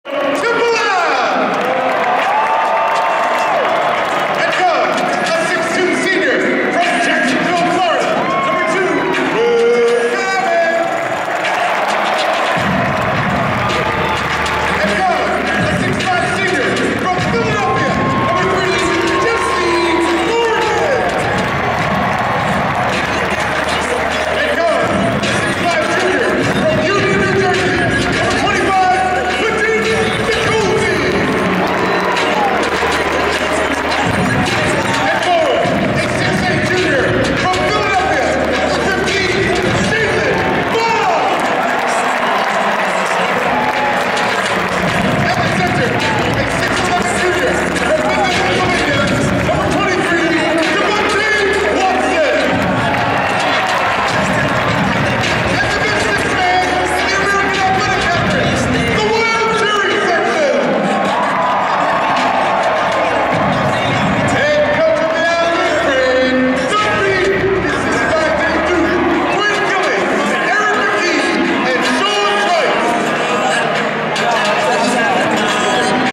Temple Owls vs. Cincinnati 2-10-15
pre-game player introductions
Owls won 75-59